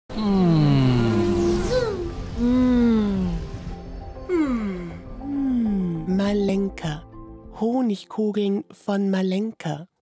Honigkugeln MARLENKA® DE
Wie genießt DU die Honigkugeln von Marlenka? 😍 Mit deiner Mama, deinen Kindern, der ganzen Familie oder einfach nur Solo? Jeder wird ihren duftigen weichen Honigteig und die köstliche Milchcreme zu schätzen wissen. Außerdem ist alles 100% frei von künstlichen Konservierungs- und Farbstoffen, wie es nur mmmMarlenka kann! 🐝